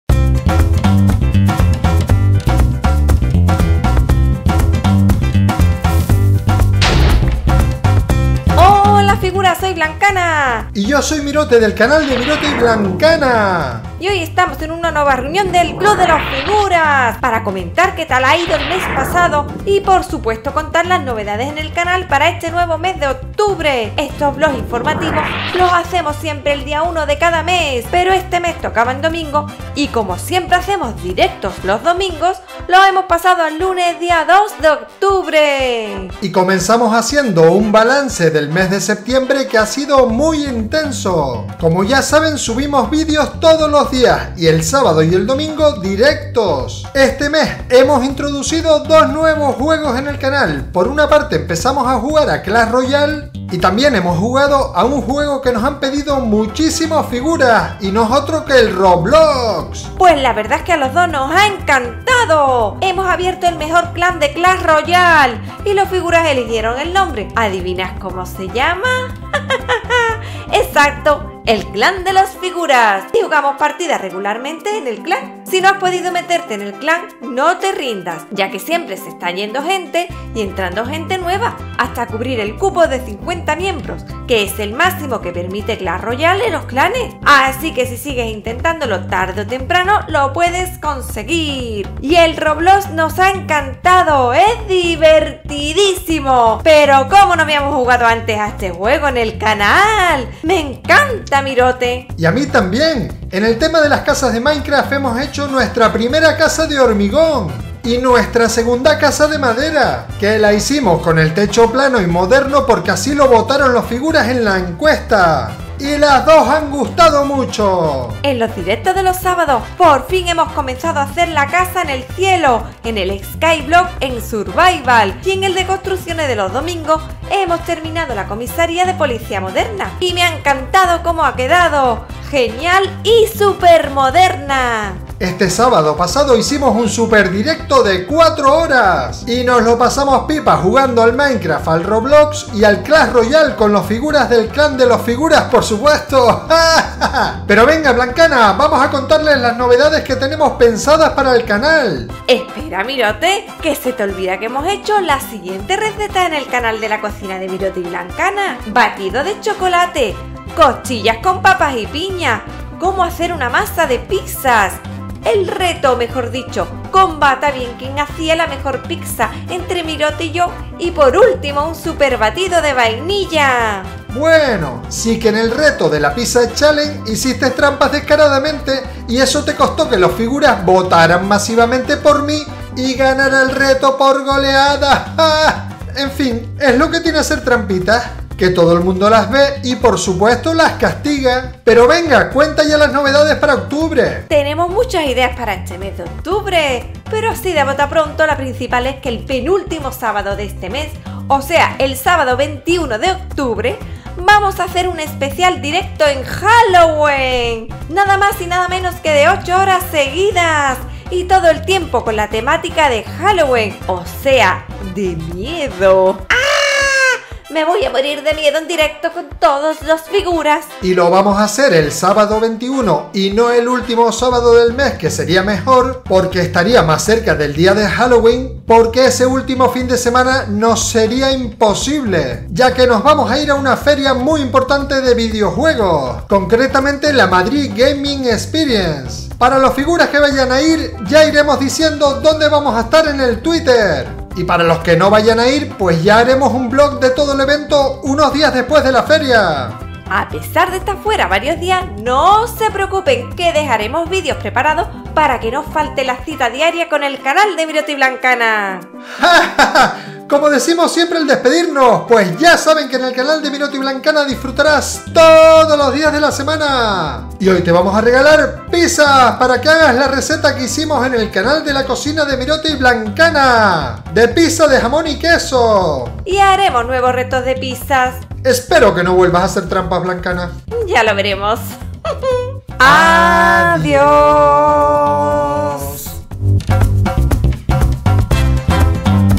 🎃NOVEDADES DE OCTUBRE EN EL CANAL
Todas las novedades que tendremos en Octubre en el canal entre ellas un directo Especial Halloween de 8 horas en minecraft, roblox y clash royale. CANAL DE MIROTE Y BLANCANA.
►¡¡LA COCINA DE MIROTE Y BLANCANA!!!